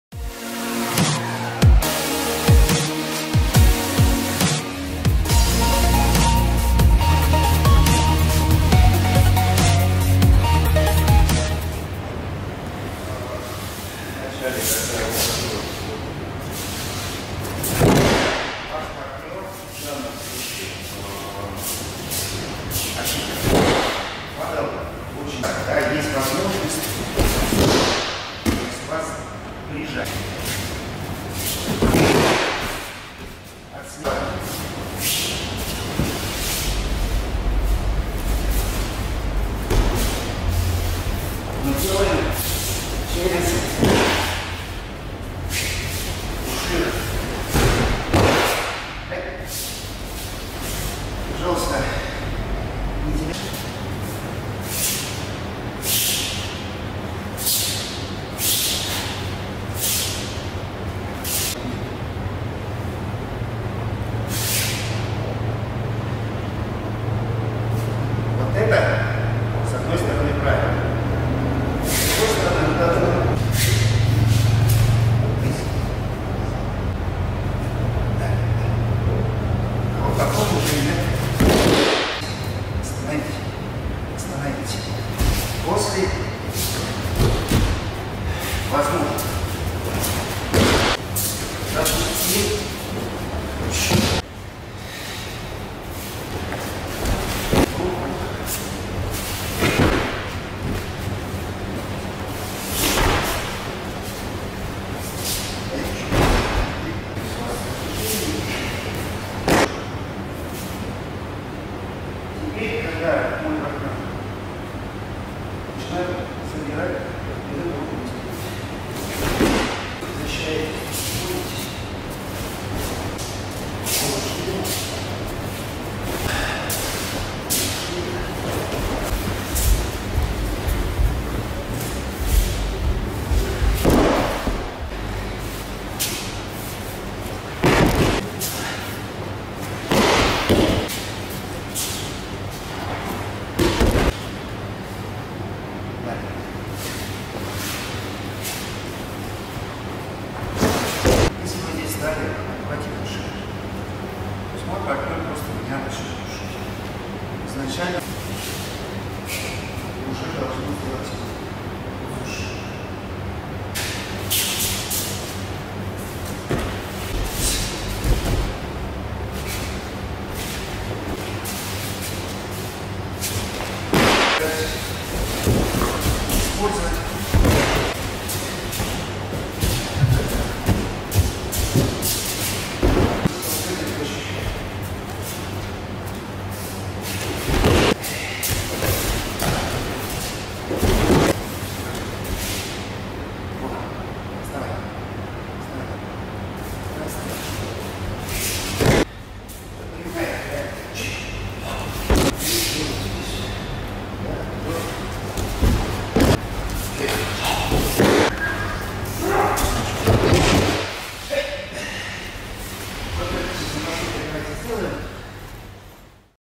Таврида Арт - Айкидо - Корнев Виталий
Корнев Виталий, 5 дан Айкидо Айкикай.
Эксперт Арт-кластера «Таврида», боевые искусства 2022